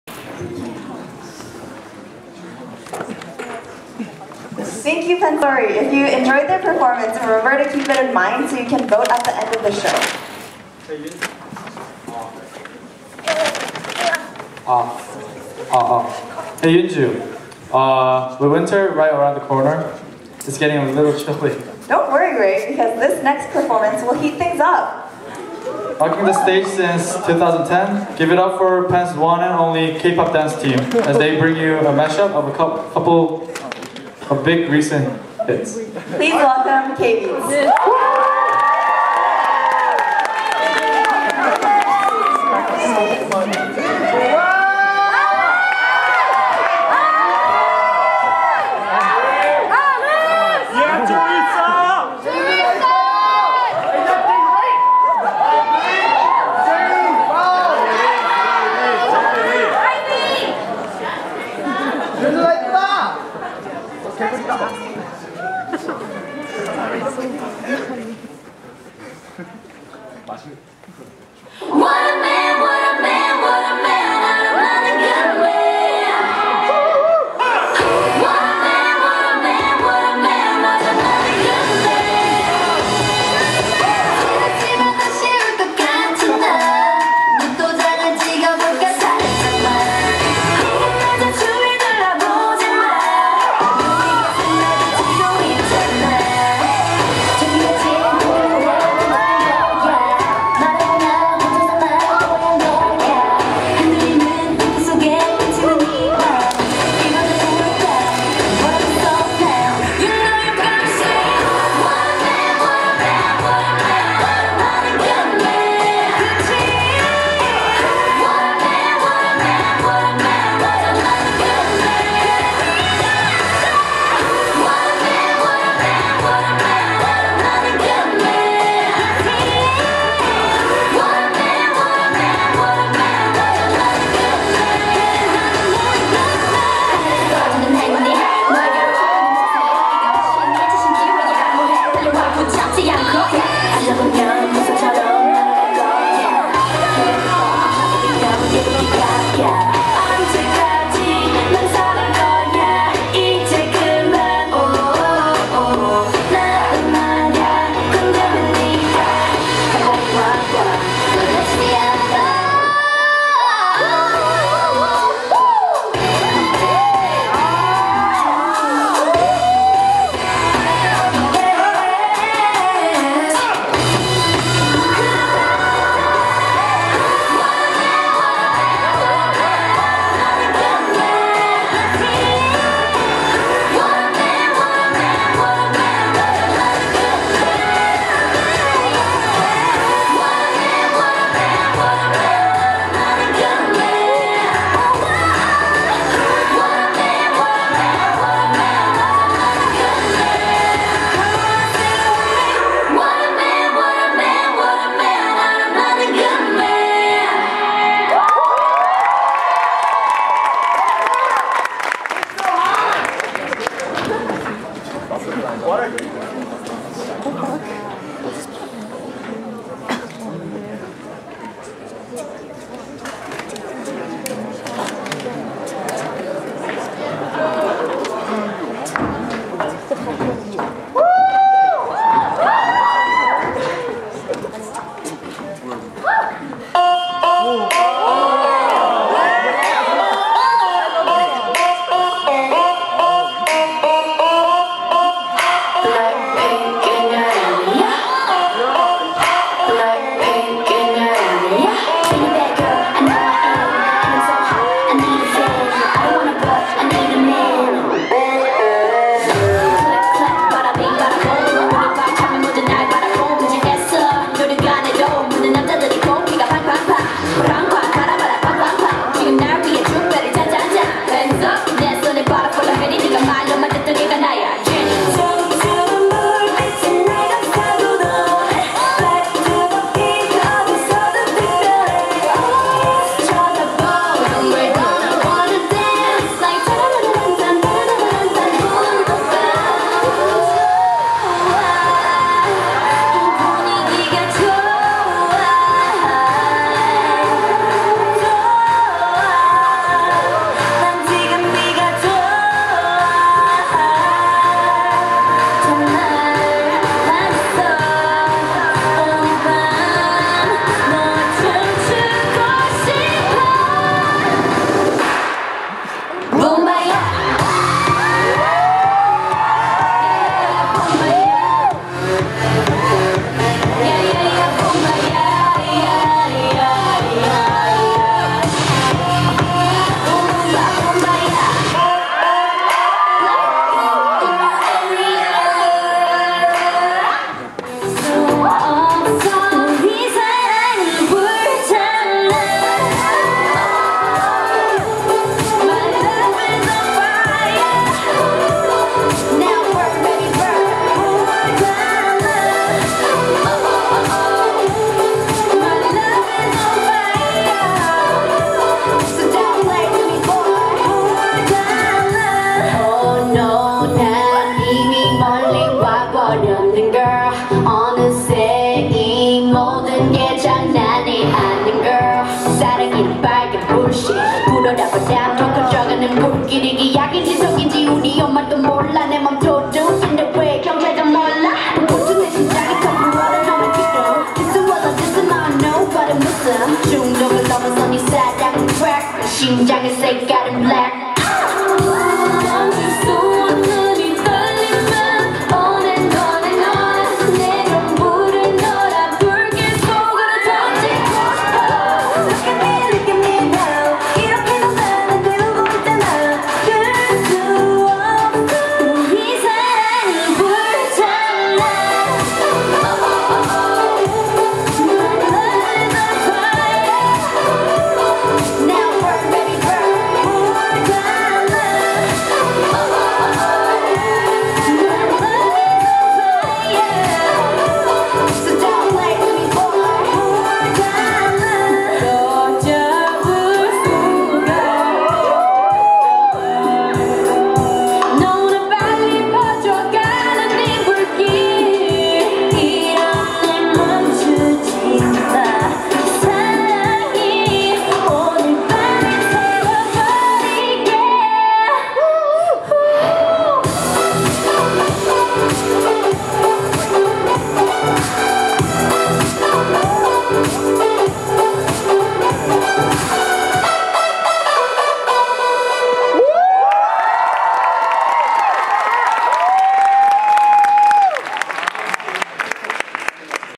KBEATS (1/2) - UPENN KPOP NIGHT 2016
Every year the University of Pennsylvania's Korean Student Association and Koreans at Penn team up to host Kpop Night, an evening of music to promote Korean culture.

Performance by kBeats